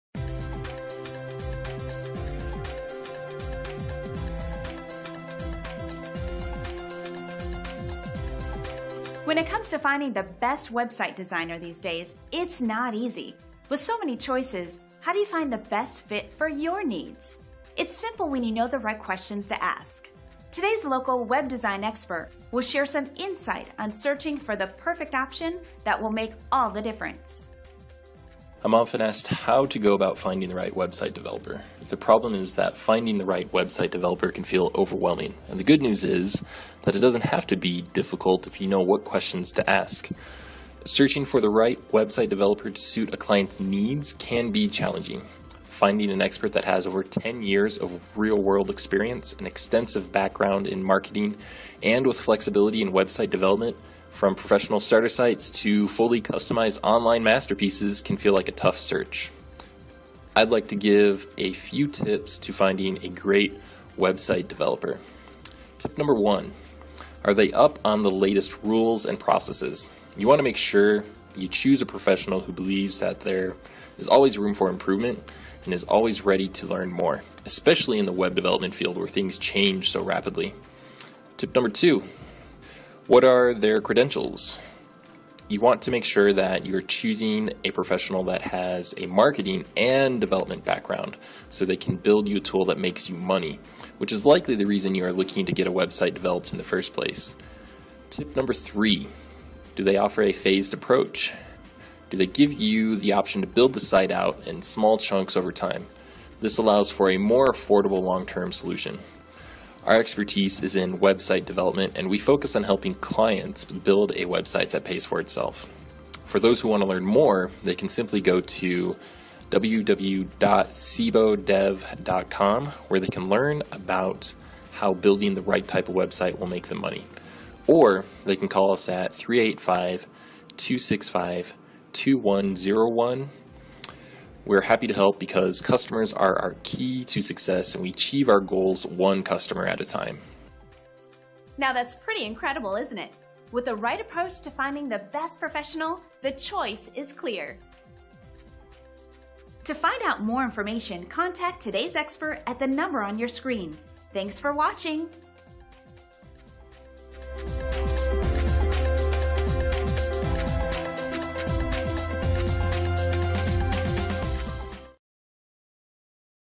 Best Website Development Provo | Website Development in Provo Utah with Peter Rakozy of Sebo Dev
Website Development in Provo Utah with Peter Rakozy of Sebo Development LLC. Peter has a passion for listening to his clients needs and helping them get their business on the internet. His knowledge and real world experience are the reason he is so effective at Website Development. To learn more about Peter Rakozy and how he can help with growing your business visit His or give him a call at he is happy to help because he achieves his goals one client at a time.